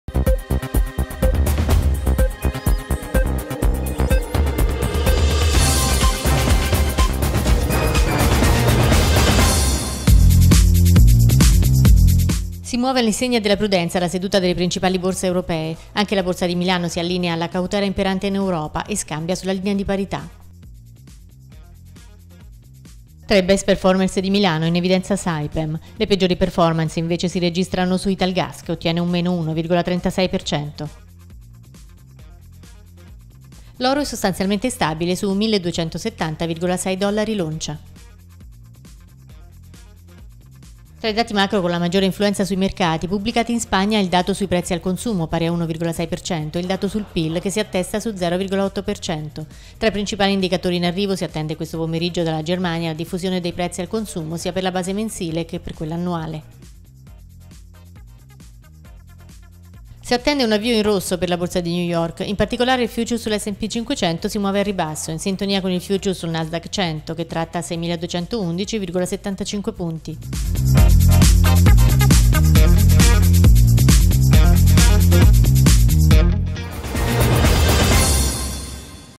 1 minuto in Borsa 30 ottobre 2017
Si muove all'insegna della prudenza la seduta delle principali borse europee. Anche la borsa di Milano si allinea alla cautela imperante in Europa, e scambia sulla linea di parità.

Tra i best performers di Milano, in evidenza Saipem (+4,08%).
Le peggiori performance, invece, si registrano su Italgas, che ottiene -1,36%.

L'Oro è sostanzialmente stabile su 1.270,6 dollari l'oncia.

Tra i dati macro con la maggiore influenza sui mercati, pubblicati in Spagna il dato sui Prezzi al Consumo, pari a 1,6%, e il dato sul PIL, che si attesta su 0,8%.
Tra i principali indicatori in arrivo, si attende questo pomeriggio dalla Germania la diffusione dei Prezzi al Consumo, sia per la base mensile che per quella annuale.

Si attende un avvio in rosso per la Borsa di New York. In particolare, il future sull'S&P-500 si muove al ribasso, in sintonia con il future sul Nasdaq 100 che tratta a 6.211,75 punti.